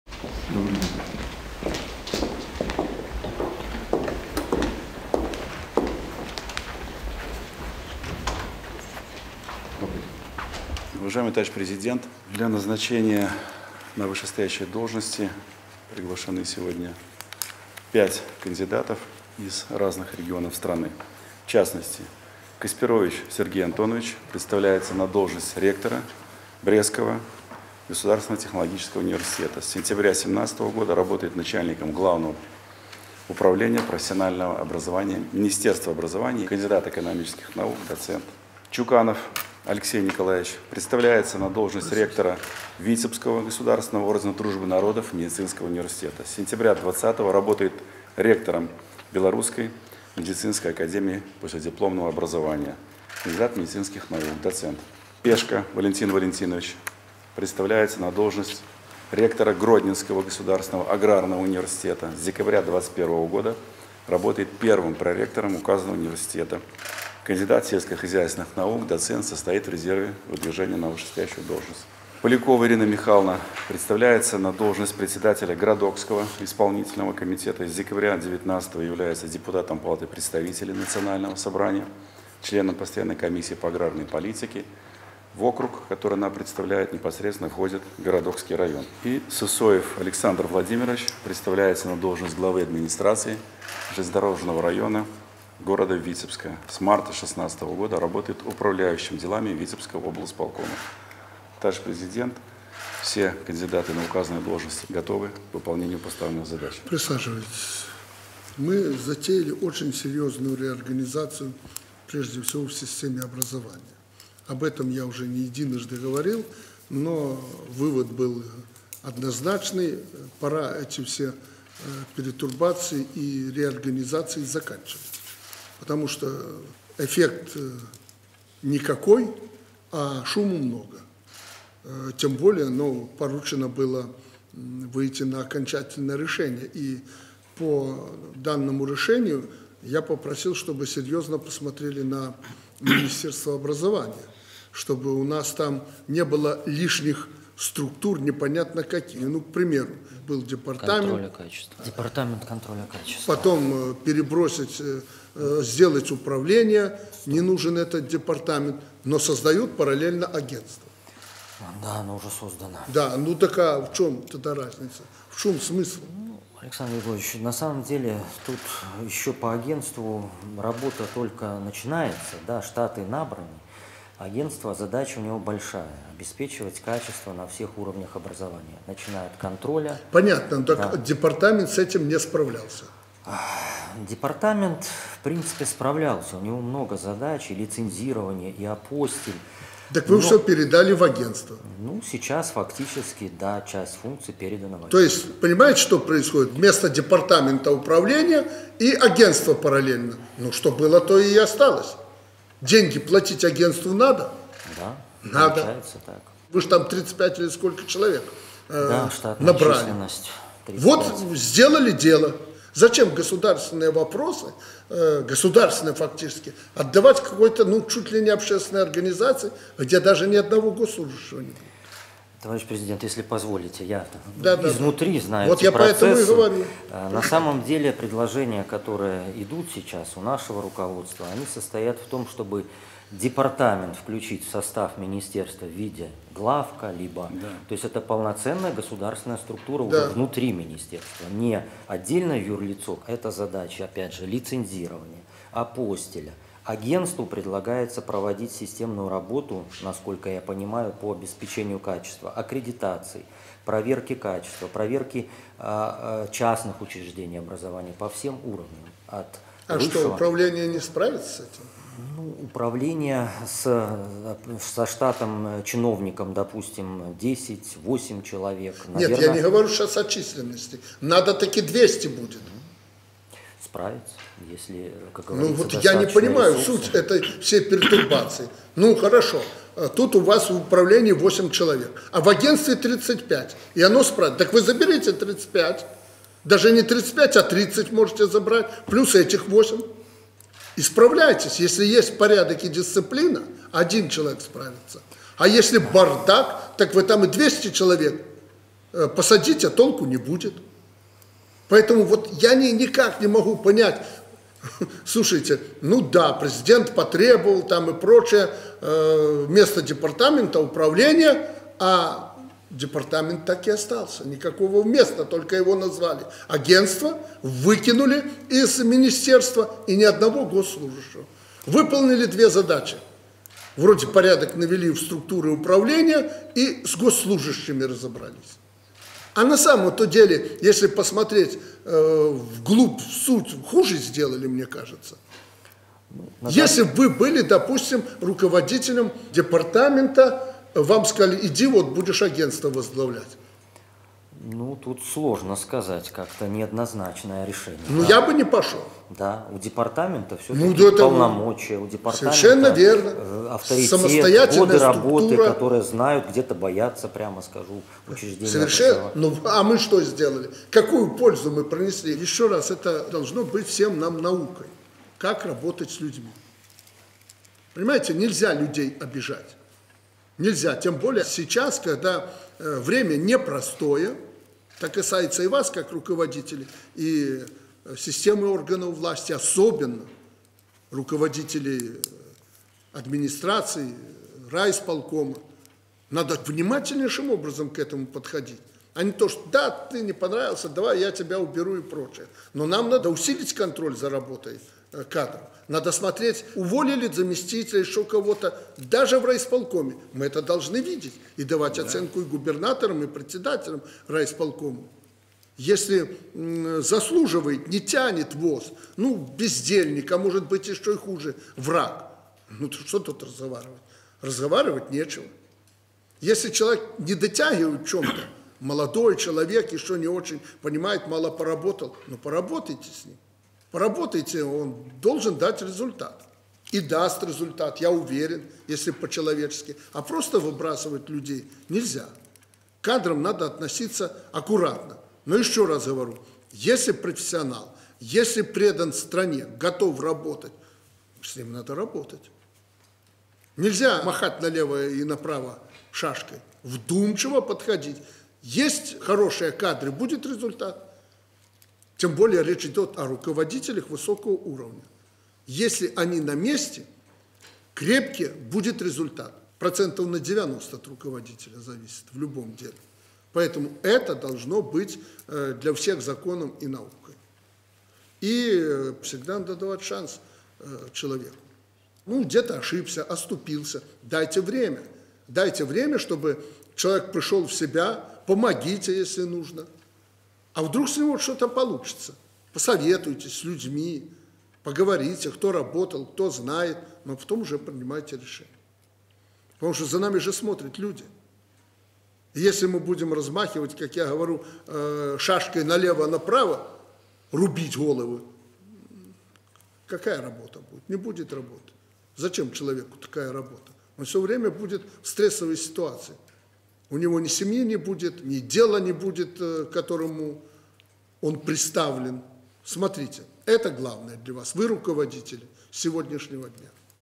Лукашенко: Ну, так а в чём тогда разница, в чём смысл? // Кадровый день
Лукашенко: Что было, то и осталось. Деньги платить агентству надо? Надо. Платить надо людям. Вы же там 35 или сколько человек набрали. Вот сделали дело!

Президент Беларуси Александр Лукашенко 17 января рассмотрел кадровые вопросы.